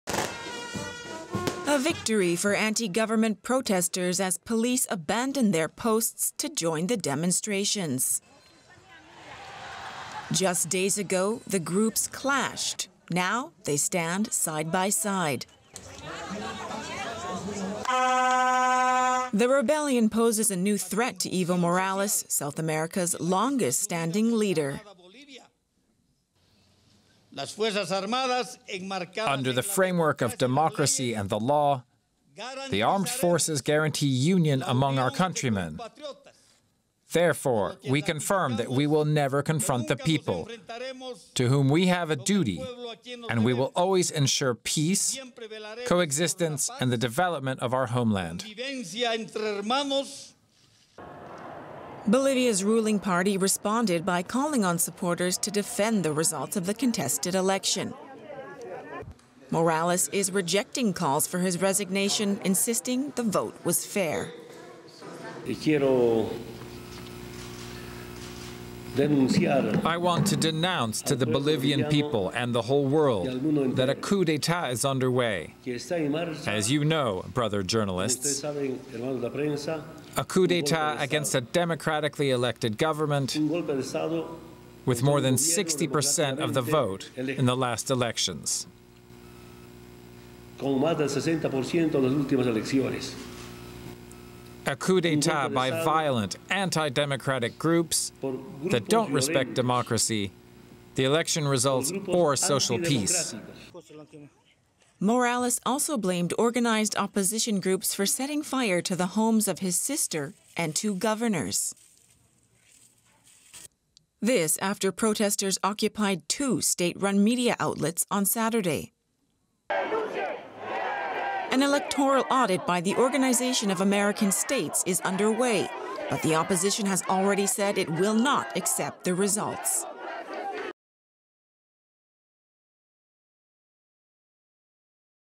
Police in Bolivia side with protesters against Morales | DW News
Demonstrators in Bolivia have occupied the headquarters of two state broadcasters and other buildings, in what President Evo Morales says is an attempted coup.  Morales is calling for talks with the opposition, but leaders have rejected his offer. They accuse him of rigging the last election. Meanwhile police officers have also changed sides, marching in the demonstrations they were meant to be bringing under control.